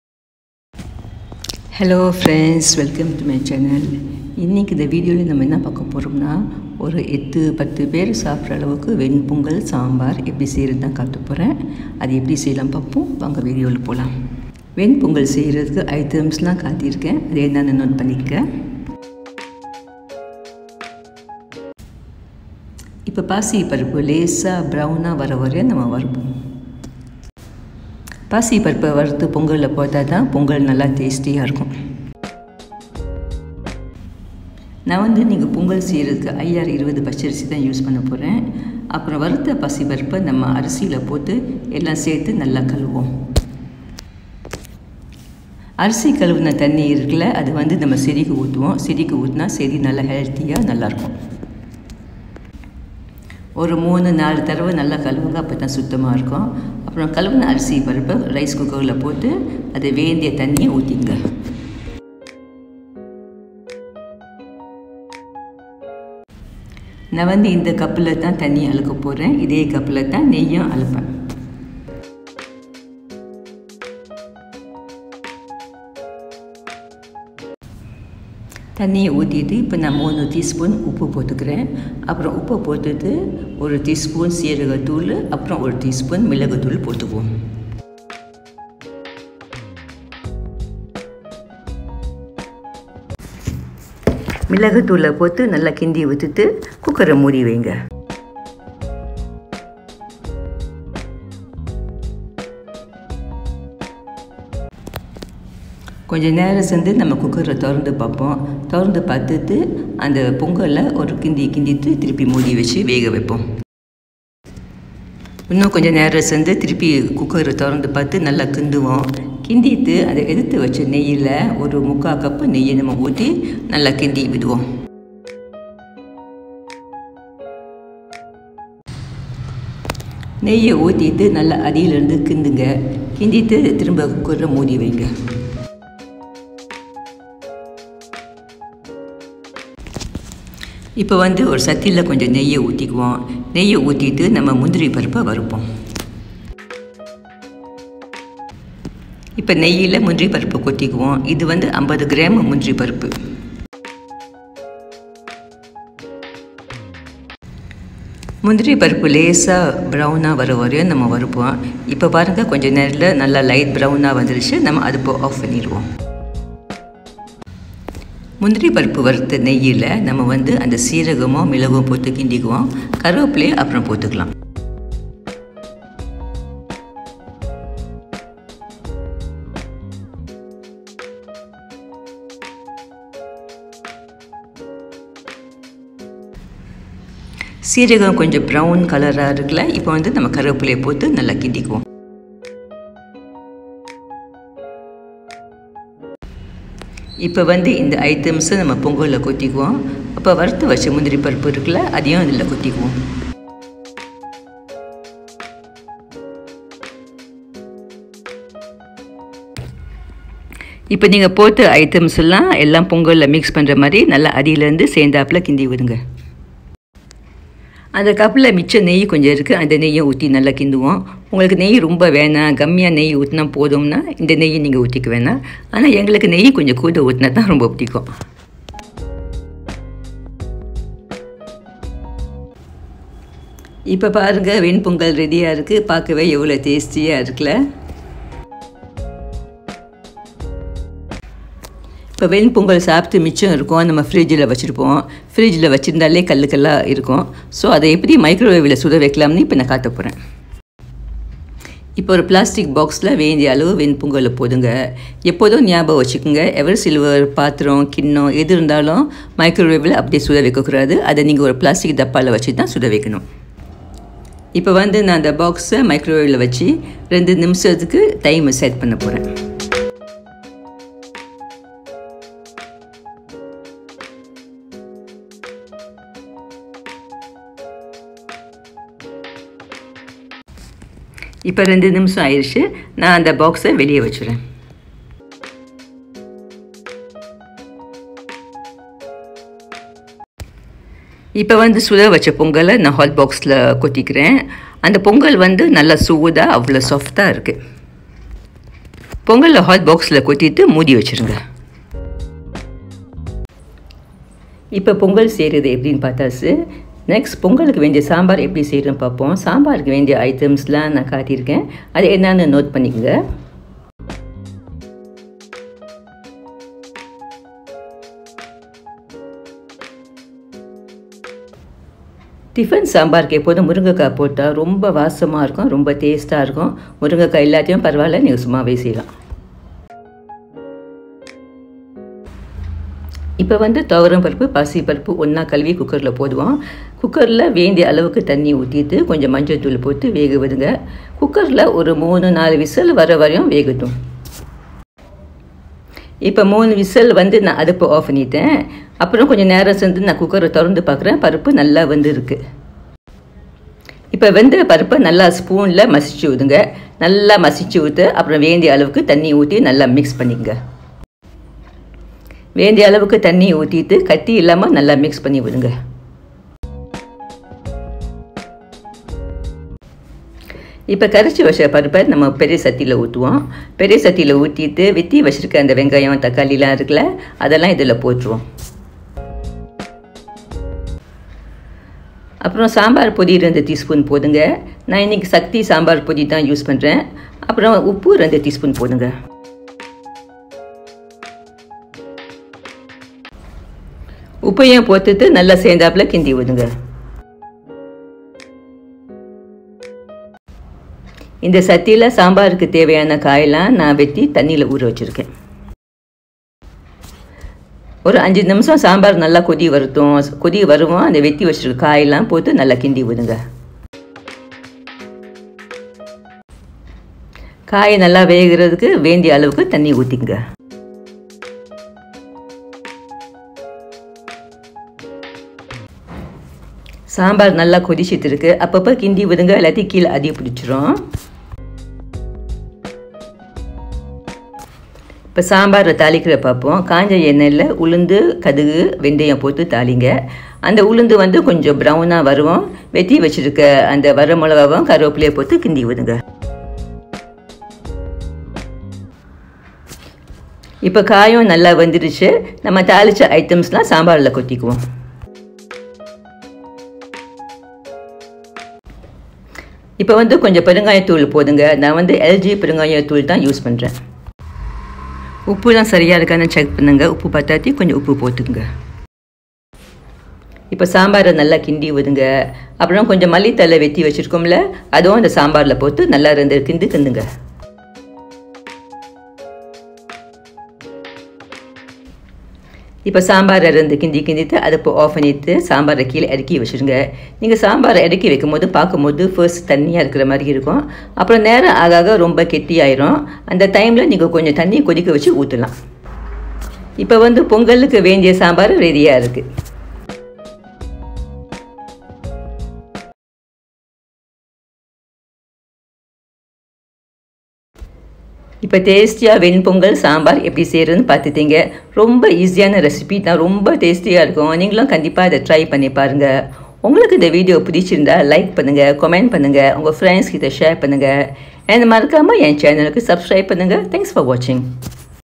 பொங்கல் & சாம்பார் / 8-10 பேர் தாராளமாக சாப்பிடலாம்/Pongal & Sambar for 8 -10 People/ September 2023
Ven Pongal & Sambar is a traditional breakfast item in S.India.It is very healthy and delicious too.It is also a favourite item in all weddings and important
functions in the Southern states of India.